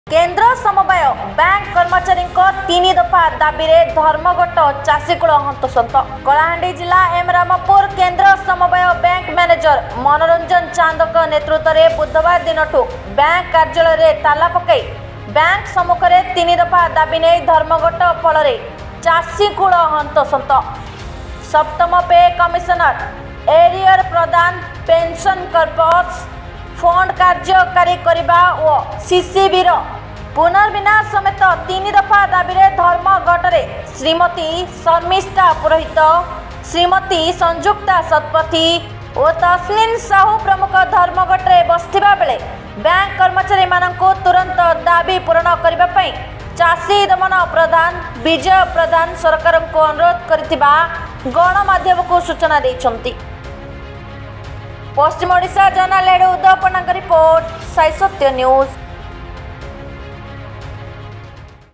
କେନ୍ଦ୍ର ସମବାୟ ବ୍ୟାଙ୍କ କର୍ମଚାରୀ ଙ୍କ ୩ ଦଫା ଦାବିରେ ଧର୍ମଘଟ ଚାଷୀ କୂଳ ହନ୍ତ ସନ୍ତ||SAISATYANEWS
saisatyanews saisatyanewsodia saisatyanewsodisha ||କେନ୍ଦ୍ର ସମବାୟ ବ୍ୟାଙ୍କ କର୍ମଚାରୀ ଙ୍କ ୩ ଦଫା ଦାବିରେ ଧର୍ମଘଟ ଚାଷୀ କୂଳ ହନ୍ତ ସନ୍ତ||Central Cooperative Bank employees are on strike with three demands